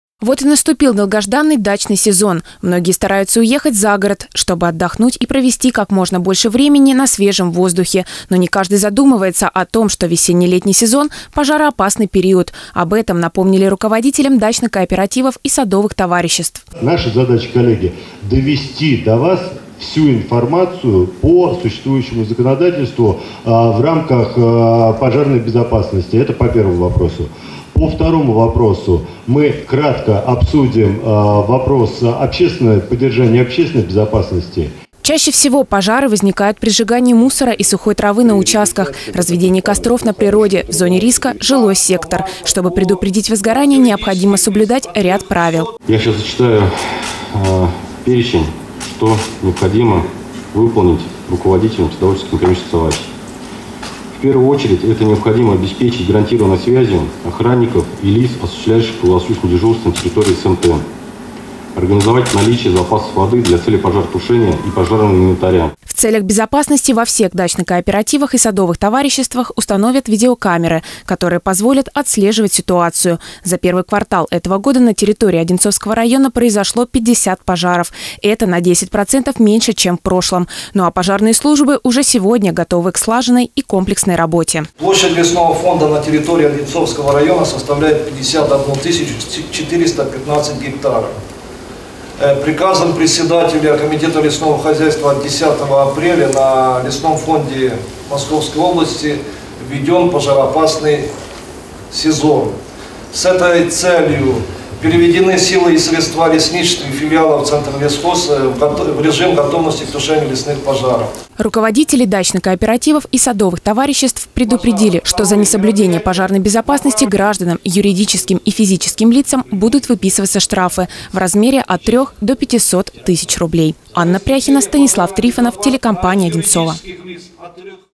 Собрание руководителей дачных кооперативов и СНТ
Участники встречи обсудили выработку и реализацию практических мер по предупреждению и ликвидации природных пожаров в 17 году. Присутствующим еще раз напомнили о важности и необходимости соблюдения правил пожарной безопасности.